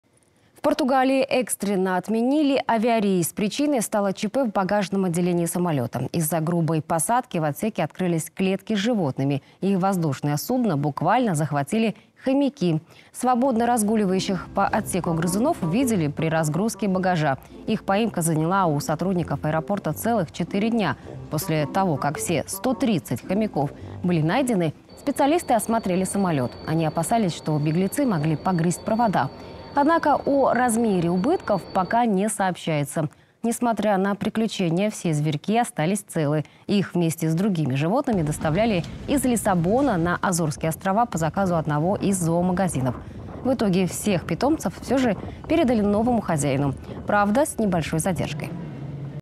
В Португалии 130 хомяков захватили самолет
В Португалии экстренно отменили авиарейс. Причиной стало ЧП в багажном отделении самолета. Из-за грубой посадки в отсеке открылись клетки с животными. И воздушное судно буквально захватили хомяки.